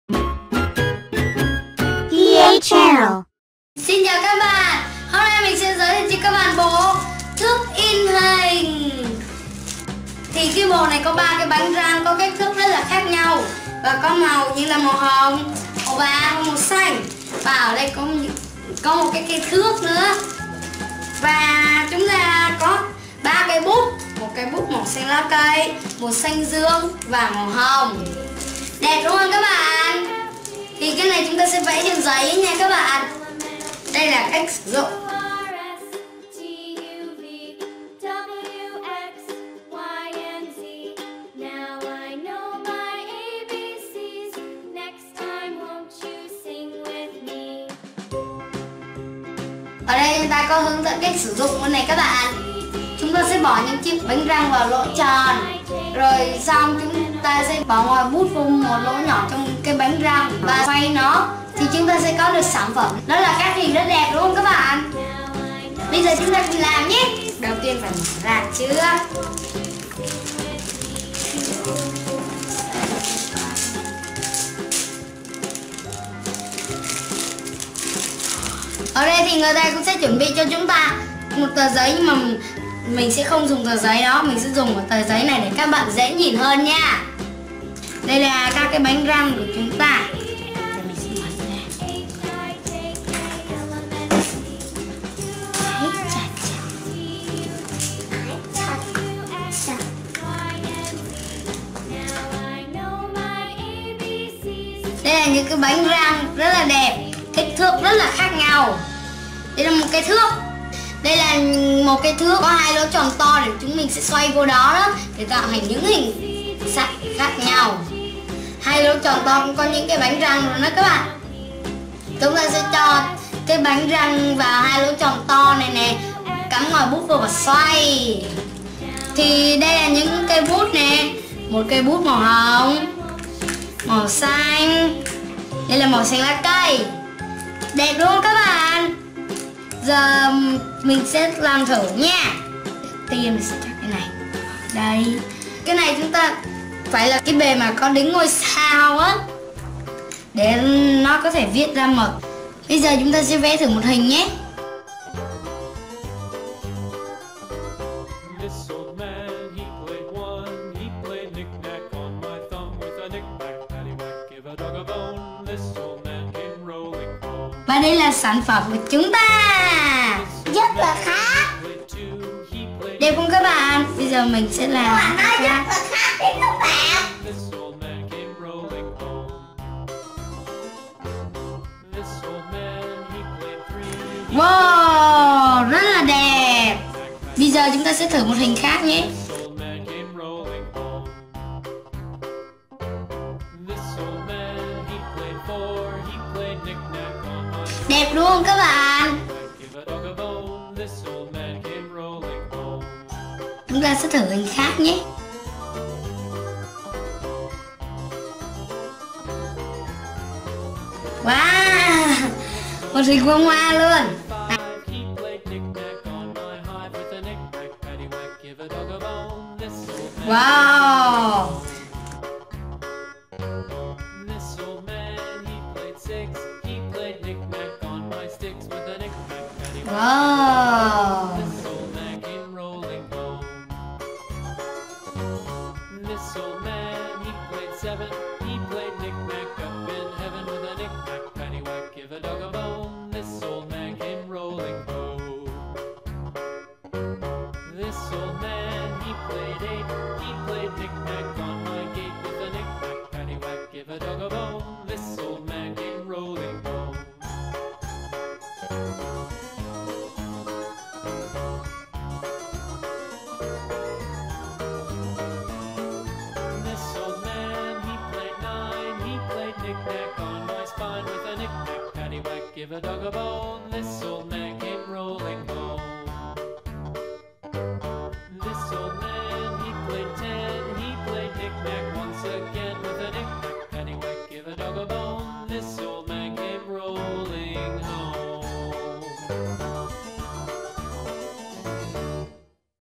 Bộ dụng cụ học tập cho trẻ em thước in hình
Bộ dụng cụ học tập độc đáo cho trẻ em thước in hình vẽ hoa văn | đồ chơi trẻ em | PA channel